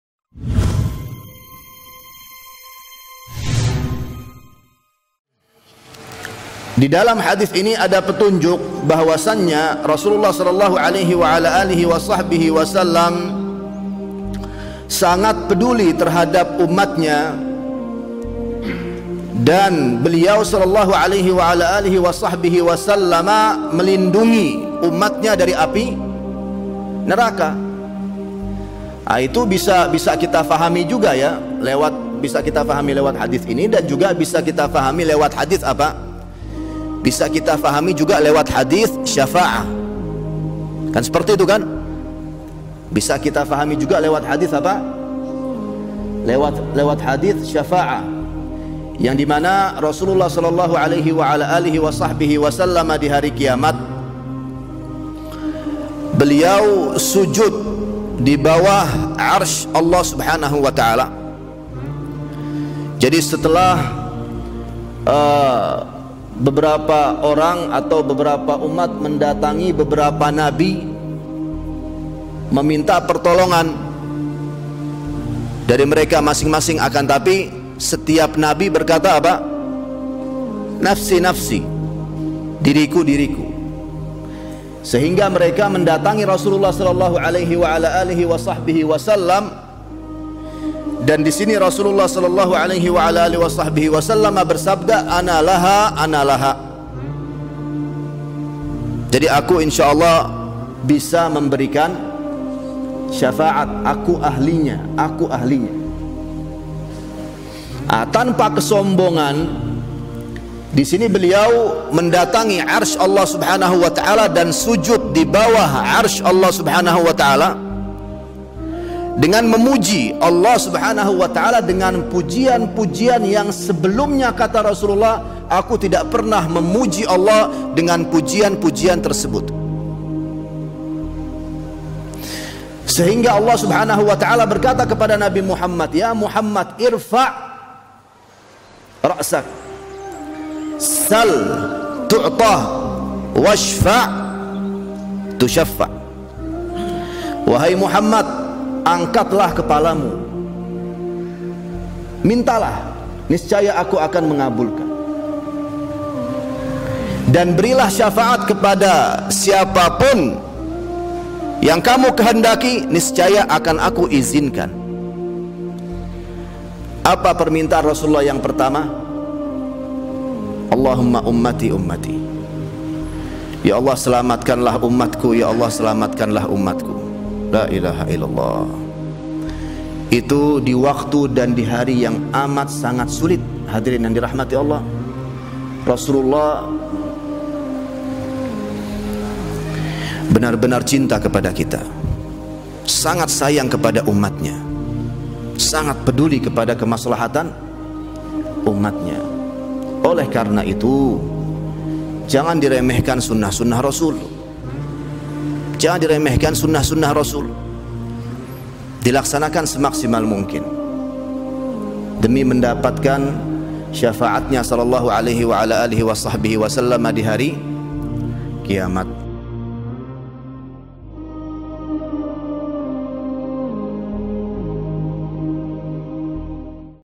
Perjuangan Nabi Muhammad Di Hadapan Allah Untuk Ummatnya
Jangan pernah remehkan sunnah nabi, lakukanlah semampu kita apa yang beliau perintahkan dan jauhi apa yang beliau larang, karena rasul saw sangat sayang kepada ummatnya dan sangat berjuang agar ummatnya semua masuk surga !

Kajian Rutin Kitab Riyadhus Shalihin Masjid Abu bakr Ash-Shiddiq Kota Malang.
Pengasuh : Ustadz Farhan Thalib M.Pd.I